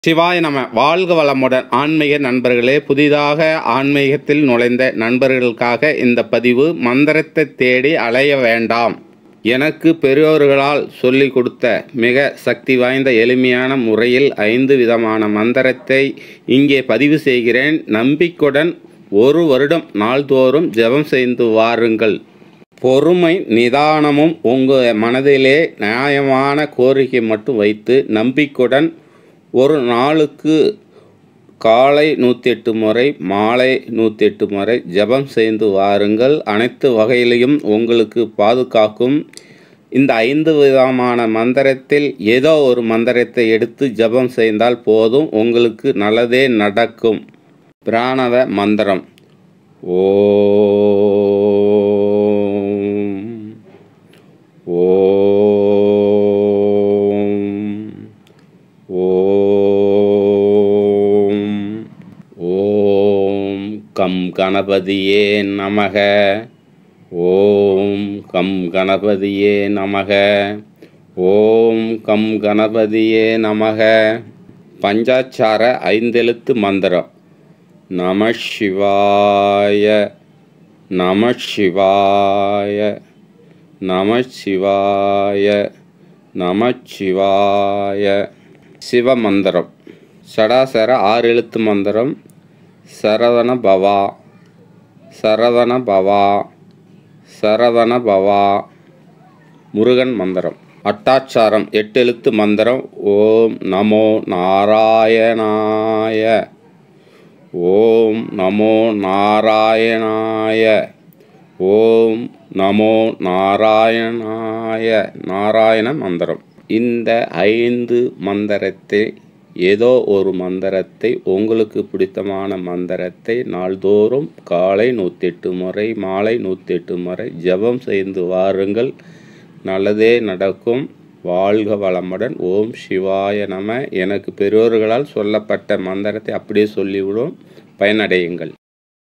எனக்கு பெரியவர்களால் சொல்லப்பட்ட ஐந்து வகையான மிக சக்தி வாய்ந்த மந்திரம்
பிரணவ-மந்திரம்
ஓம் கம் கணபதியே நமஹ
பஞ்சாச்சரம் ஐந்தெழுத்து மந்திரம்
நமசிவாய (நமச்சிவாய) சிவ-மந்திரம்

சடாச்சரம் ஆறெழுத்து மந்திரம்
சரவணபவ முருகன்-மந்திரம்

அட்டாச்சரம் எட்டெழுத்து மந்திரம்
ஓம_நமோ_நாராயணாய நாராயண-மந்திரம்